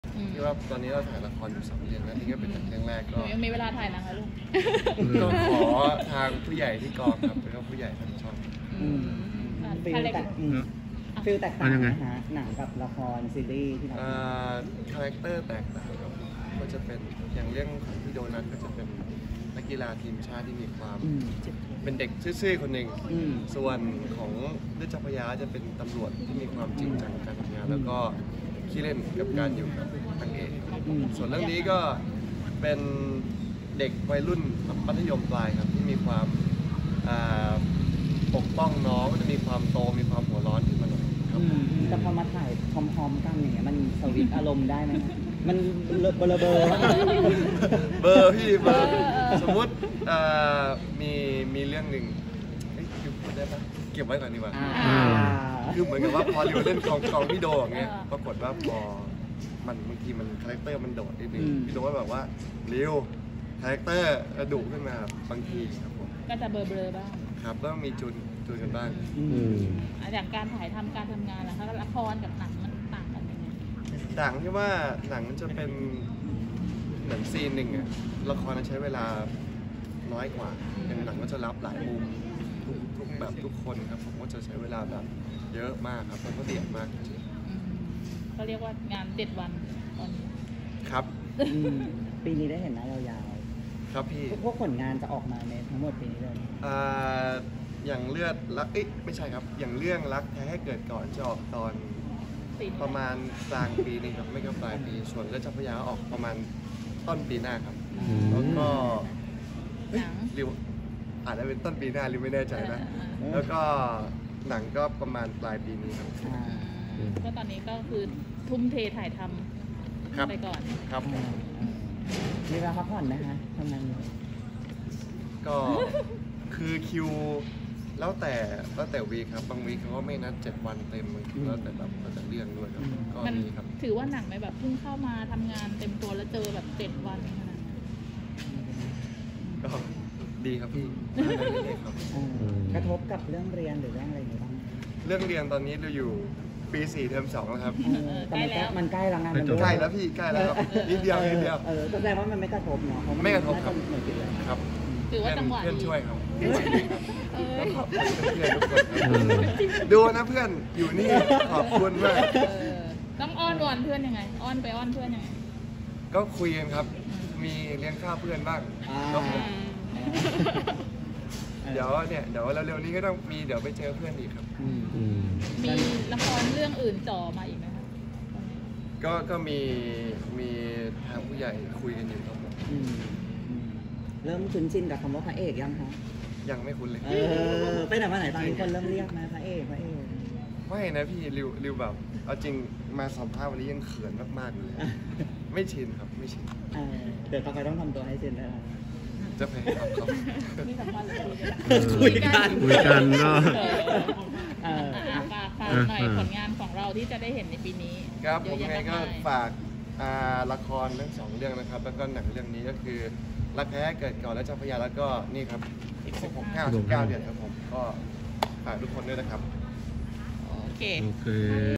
“ริว วชิรวิชญ์” ฟุ้งงานรุมสวิตซ์คาแรกเตอร์ไม่ทันถูก”โดนัท”ดุ ยังไม่ชินถูกเรียกแบบนี้
“ริว วชิรวิชญ์” ฟุ้งงานรุมสวิตซ์คาแรกเตอร์ไม่ทันถูก”โดนัท”ดุ เขินยังไม่ชินถูกเรียกแบบนี้